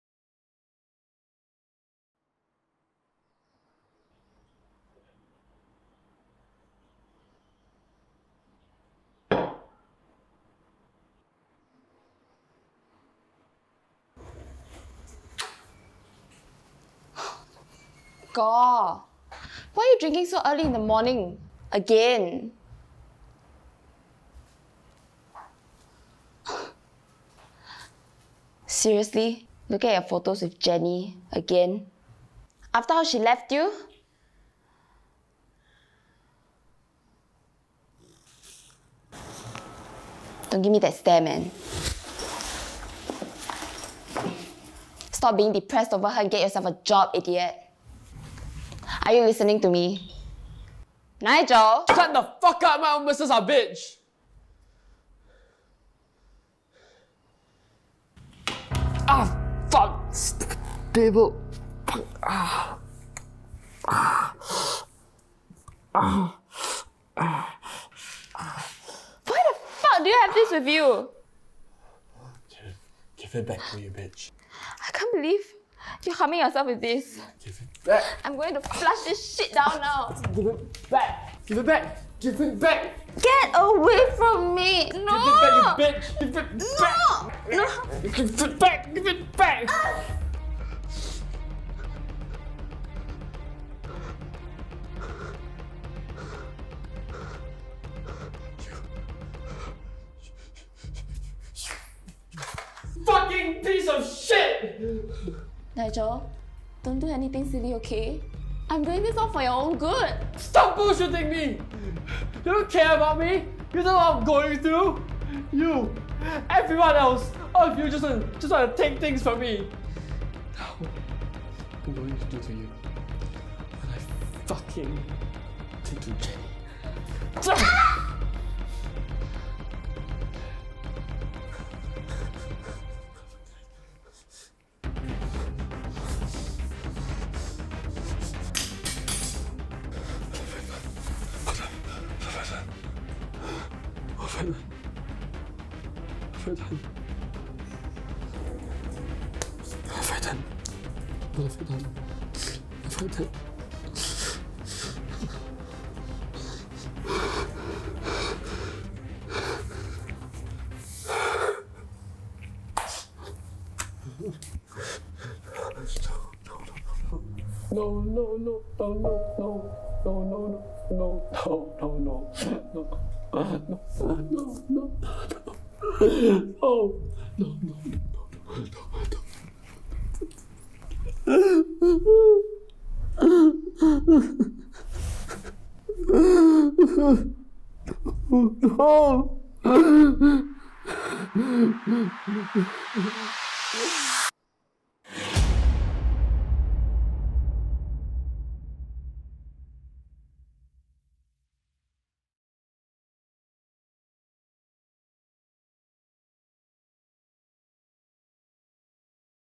Intoxikill
A short film I directed and filmed for my directing module in school.
Not a brilliant masterpiece, but I think it's a good effort.

Starring:
Nigel - Jedidiah Huang
Natalie - Chermaine Cham

Written & directed by Wong Min Quan
Director of Photography - Wong Min Quan
Production Assistant & makeup artist - Amabelle Sng
Sound Recordist - Charlotte Koh

Special thanks to my lecturer Mr Michael Kam

NOTE: this film was shot before the circuit breaker measures in Singapore were in place.